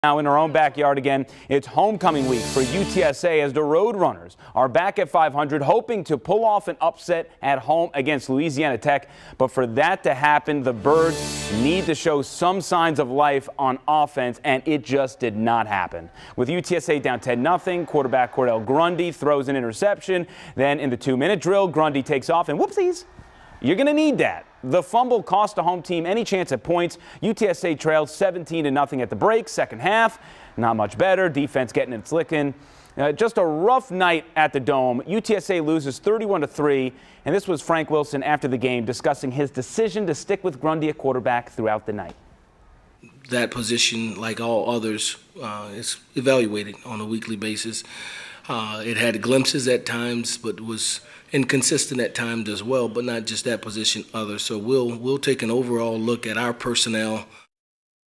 Louisiana Tech dominates UTSA at the Alamodome
The UTSA Roadrunners lost to the Louisiana Tech Bulldogs 31-3 on Saturday, October 13, 2018 at the Alamodome.